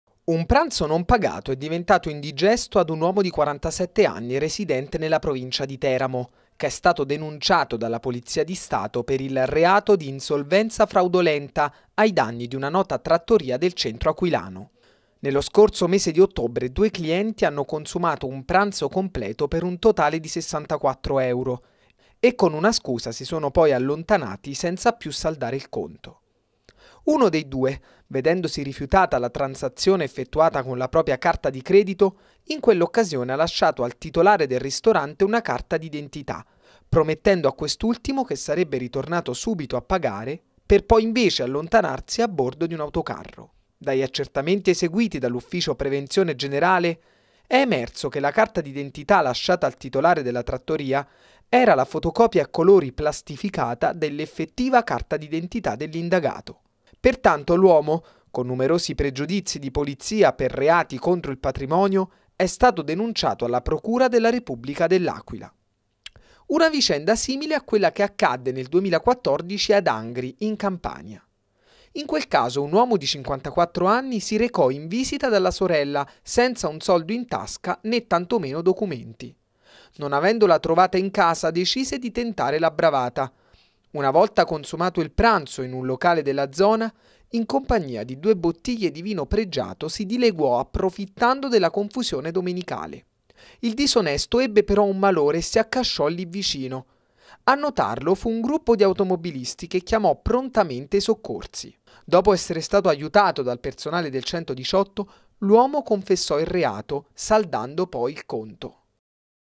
L'Aquila. Denunciato per non aver pagato il pranzo
Un uomo residente in provincia di Teramo è stato denunciato per aver eluso il pagamento di un pranzo in una nota trattoria aquilana